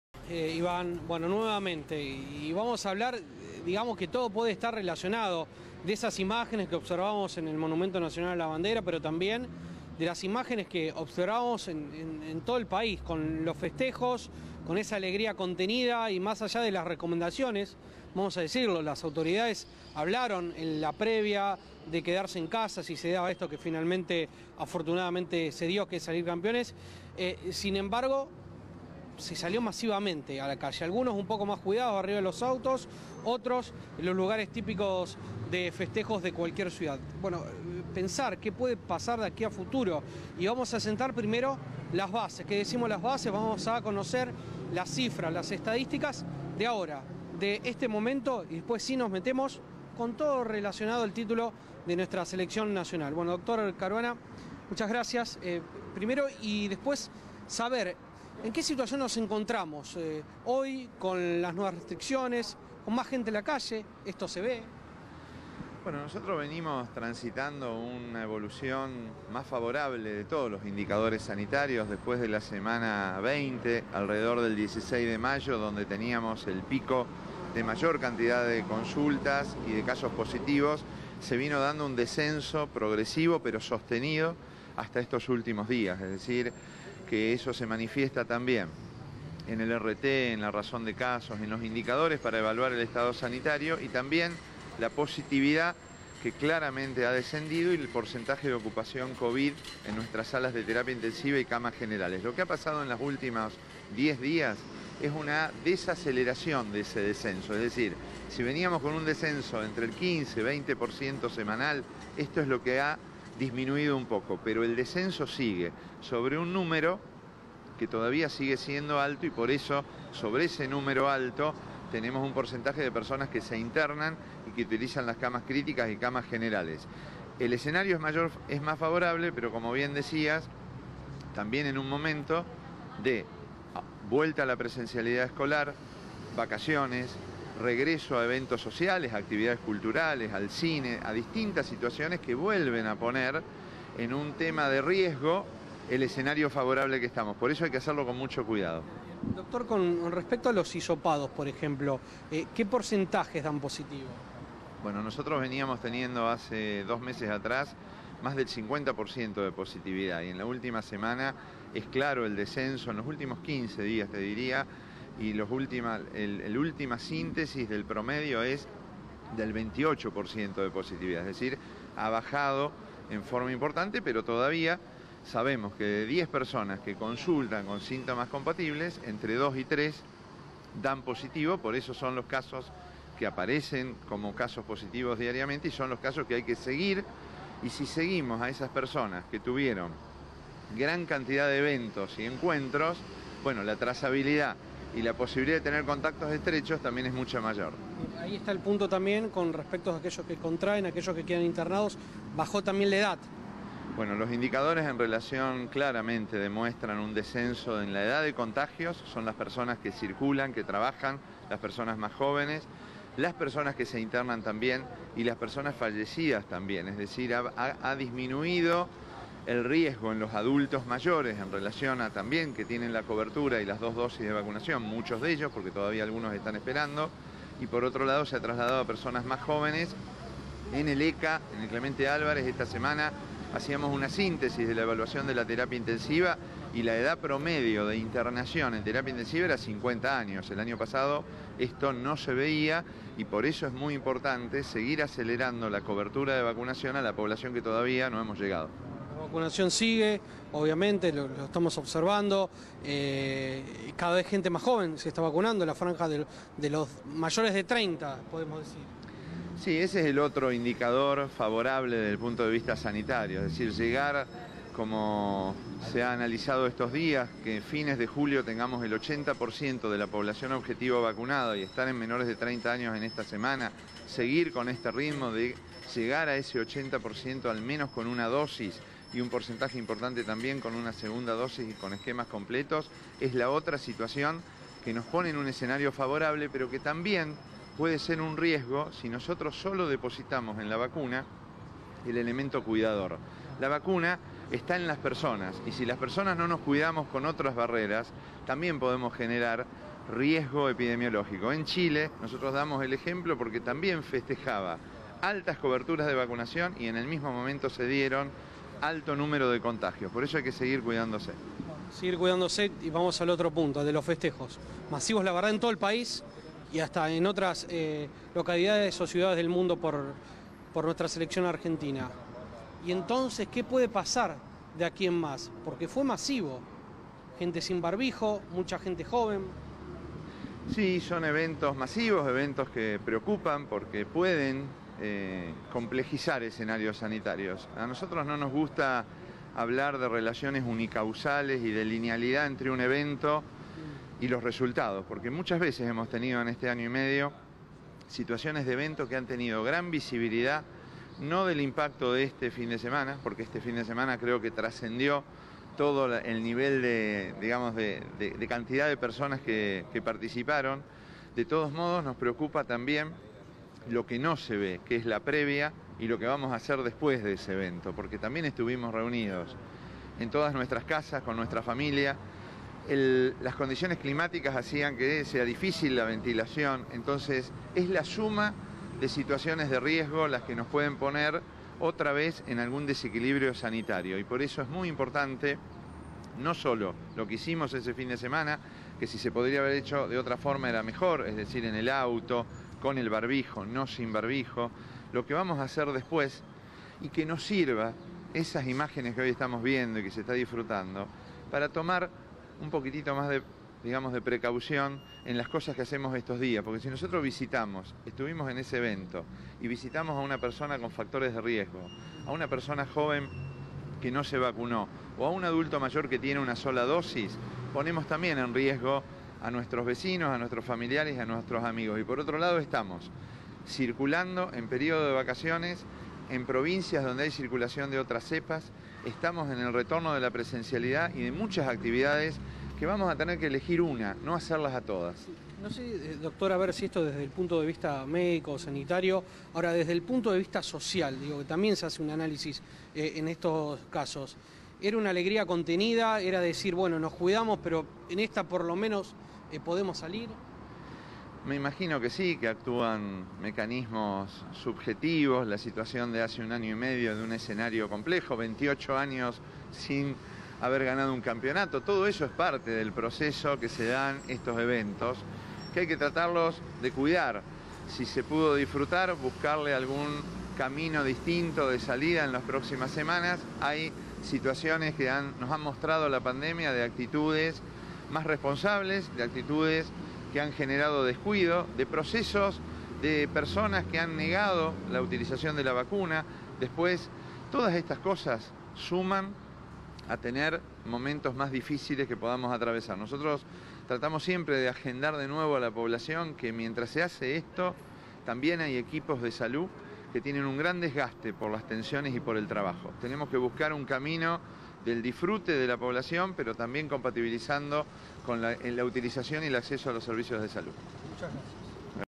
Preocupación sanitaria por los festejos de Argentina
Cómo afectan las celebraciones a los contagios por Covid-19